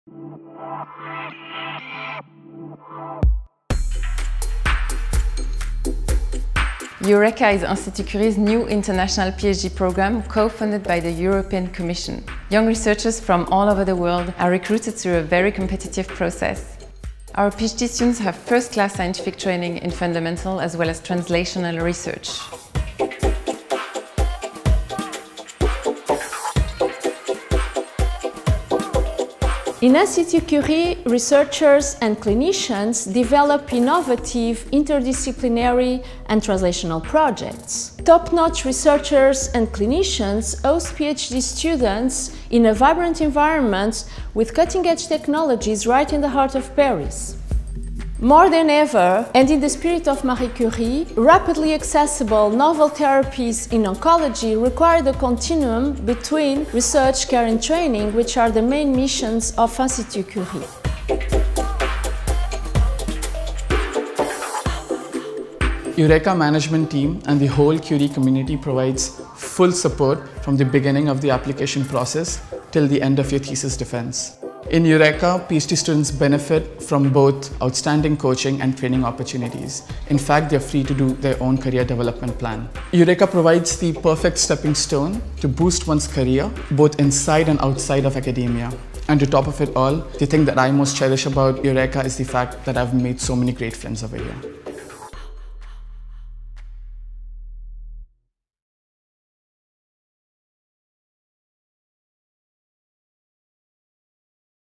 Programme doctoral EuReCa - The EuReCa PhD Program
Co-financé par la Commission européenne, EuReCa (Europe, Research & Care) est le nouveau programme doctoral international d’excellence interdisciplinaire et intersectoriel de l’Institut Curie. Pour postuler, les candidats doivent déposer leur dossier via la page web EuReCa avant le 28 janvier 2021 à 16:00 CET

EuReCa (Europe Research & Care) is Institut Curie’s international PhD Program which provides PhD students with an excellent interdisciplinary, inter-sectorial, and international training. It includes a personalized career development plan, coaching, mentoring and the possibility of undertaking secondments in partner institutions.